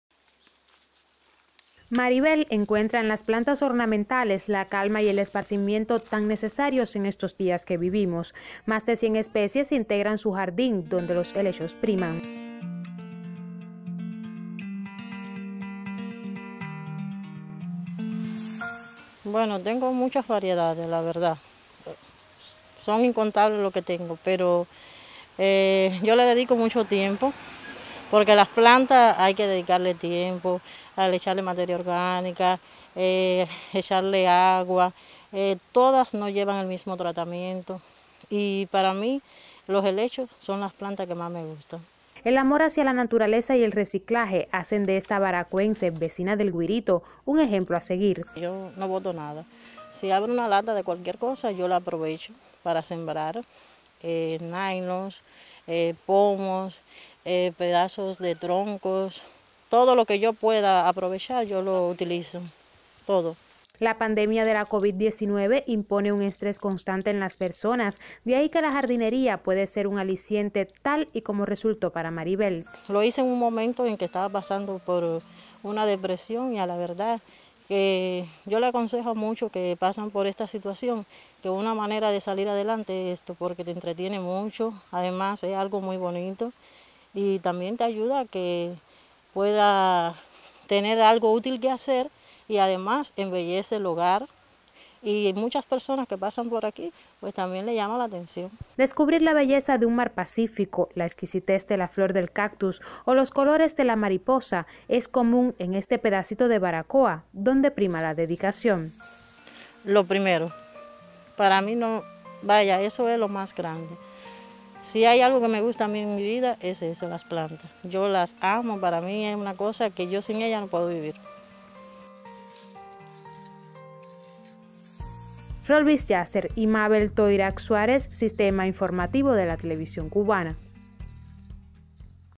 HISTORIA JARDÍN MARIBEL
Ocupar el tiempo libre en algo útil puede ser un aliciente ante el constante estrés que genera la pandemia de COVID 19. La siguiente historia así lo refleja.